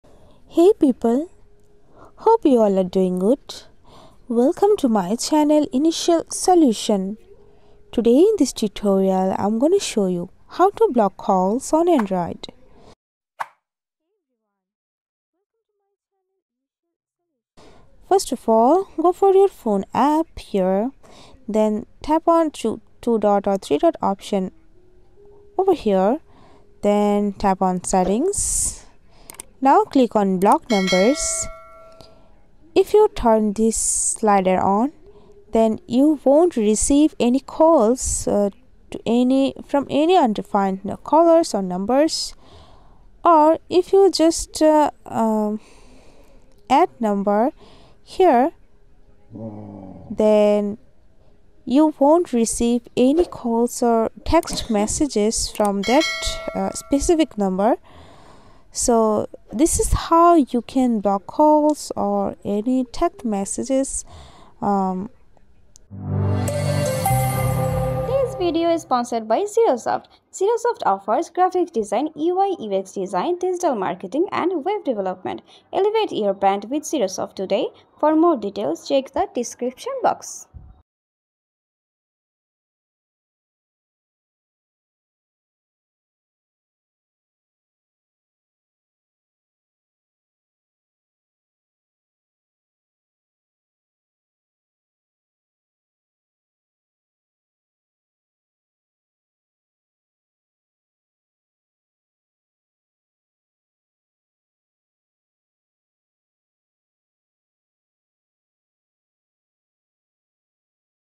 How to Block Calls on Android 2024 | Complete Tutorial
Tired of unwanted calls on your Android device? Learn how to block calls and regain control of your phone with this step-by-step guide.

📌 Step-by-Step Guide:

🚫 Blocking Calls (Steps May Vary):
Open the "Phone" app on your Android device.
Go to your "Recent Calls" or "Call History."
Find the number you want to block and tap on it.
Tap the three-dot menu or "More" options.
Select "Block/report spam" or a similar option.
Confirm the block to prevent calls from that number.

📖 Chapters of How to Block Calls on Android:
00:00- Introduction
00:07 - Tutorial Topic: How to Block Calls on Android
00:24 - Step 1: Open the Phone App
00:31 - Step 2: Access Phone Settings
00:47 - Step 3: Tap on "Block Numbers"
00:57 - Step 4: Enable Blocking for Unknown Numbers
01:11 - Step 5: Add Specific Numbers to Block
01:23 - Conclusion and Call to Action: Like, Comment, Share, Subscribe
01:34 - Sponsorship Message from Xirosoft
01:41 - End of Video